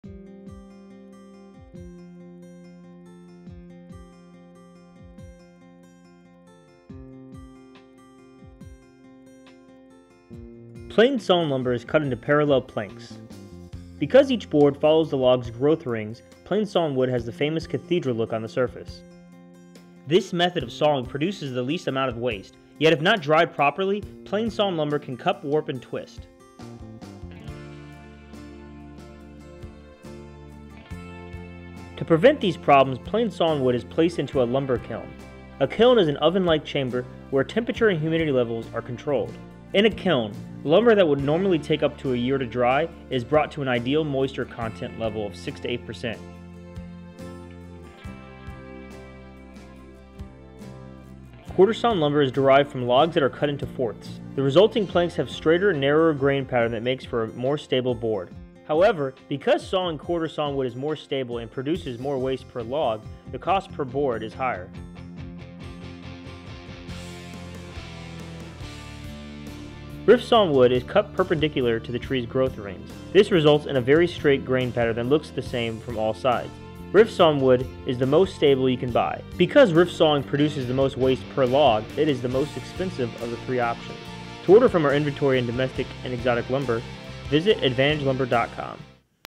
What Are the Types of Sawn Lumber?
Discover the differences between flat/plain sawn, quarter sawn, and rift sawn lumber. Why is one type of sawn lumber more expensive than another? You'll also find out how quarter and rift sawn lumber is more stable than plain sawn wood. Also learn why kiln drying hardwoods is so important.

Hardwood decking, deck tiles, live edge slabs, and more. We ship directly to your home or job site worldwide.